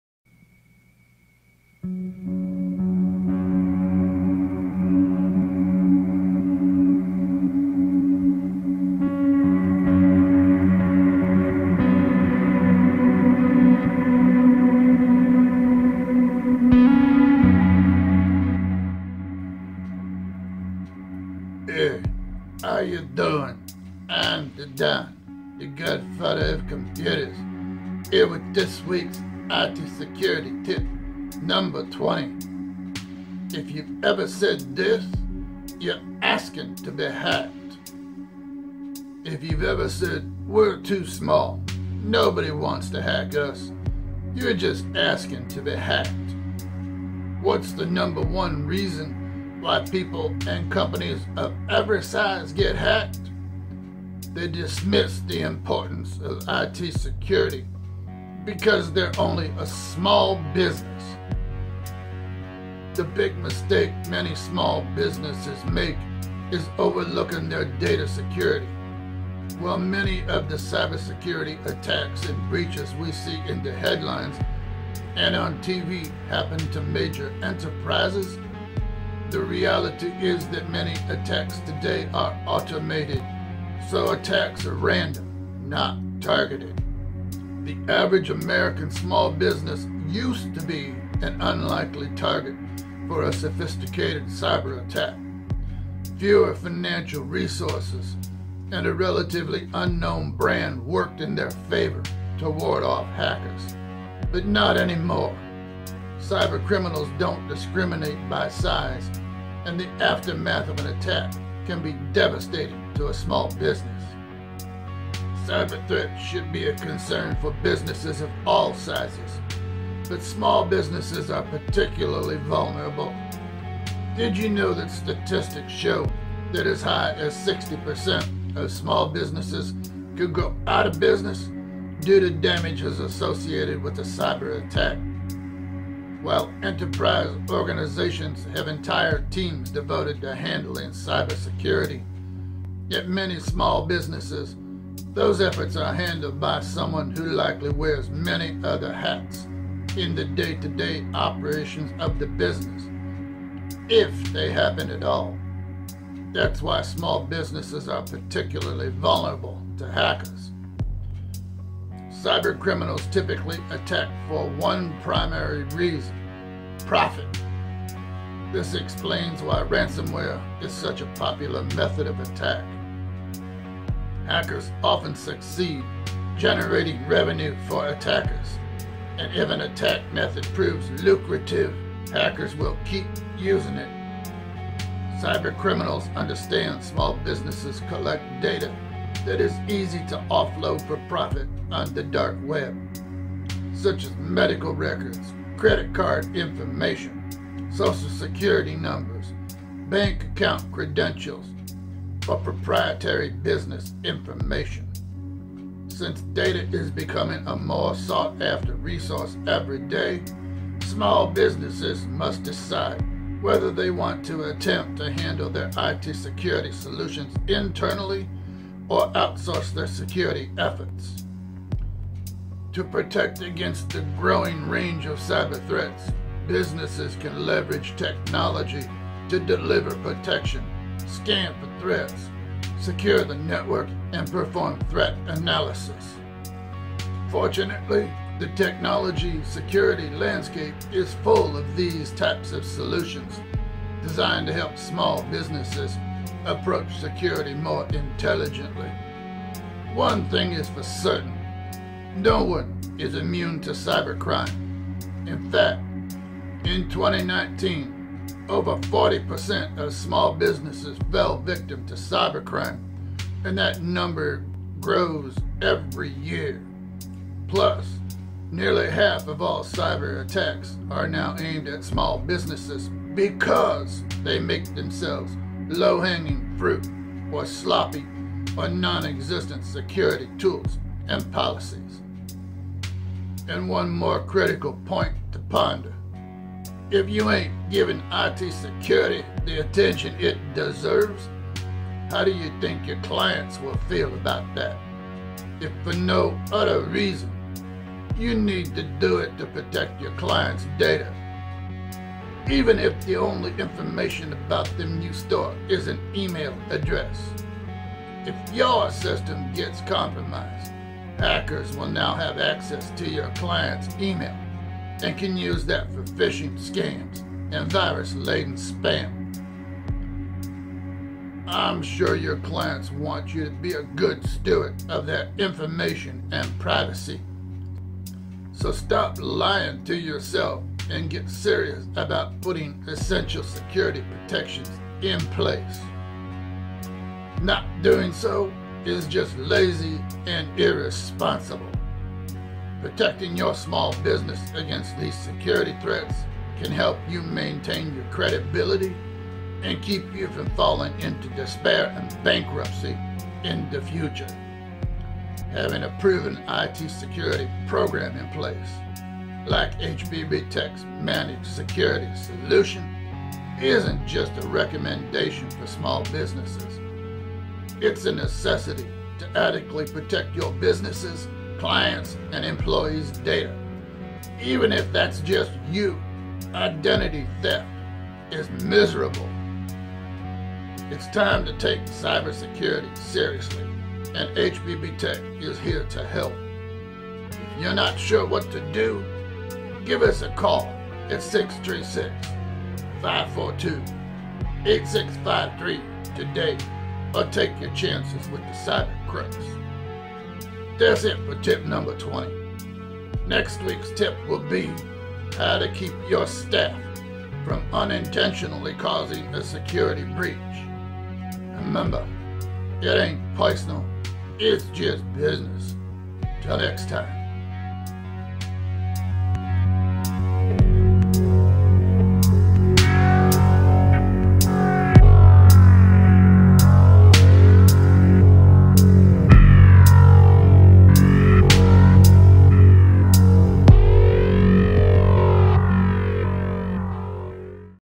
Security Tip #20 If You’ve Ever Said This, You’re ASKING To Be Hacked!
The big mistake many small businesses make is overlooking their data security. While many of the cybersecurity attacks and breaches we see in the headlines and on TV happen to major enterprises, the reality is that many attacks today are automated, so attacks are random, not targeted.

The average American small business used to be an unlikely target for a sophisticated cyberattack. Fewer financial resources and a relatively unknown brand worked in their favor to ward off hackers. But not anymore. Cyber-criminals don’t discriminate by size and the aftermath of an attack can be devastating to a small business.

Cyber threats should be a concern for businesses of all sizes, but small businesses are particularly vulnerable. Did you know that statistics show that as high as 60 percent of small businesses could go out of business due to damages associated with a cyberattack?